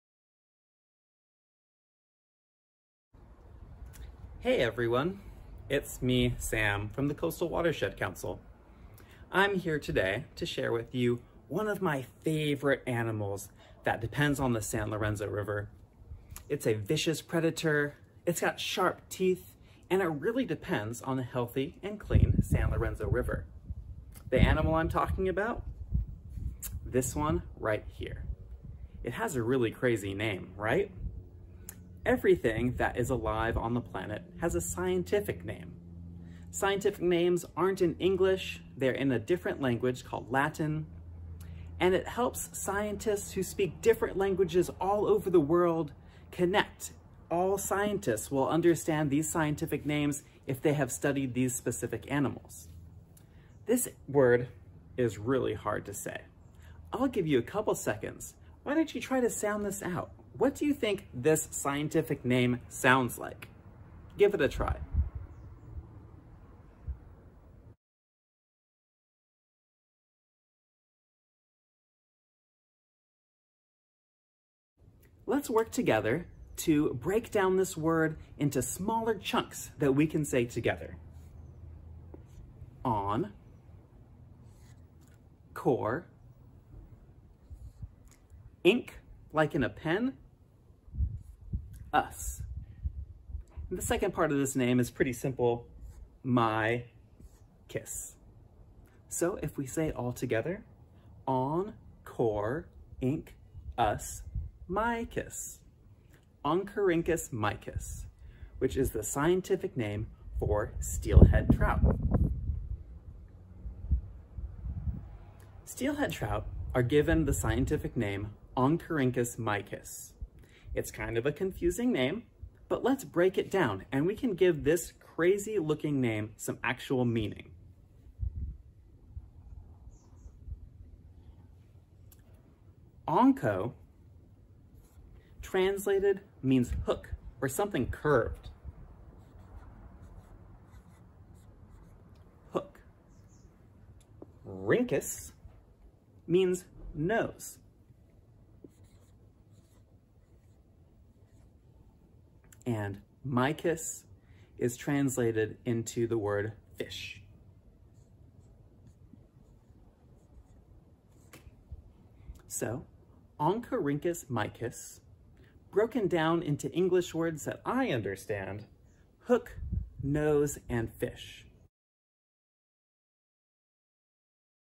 Steelhead Trout: Scientific Name (Part 1)
All living things discovered by scientists are given a scientific name, including steelhead trout! Let's learn about the scientific name of this incredible fish living in the San Lorenzo River and how to pronounce it.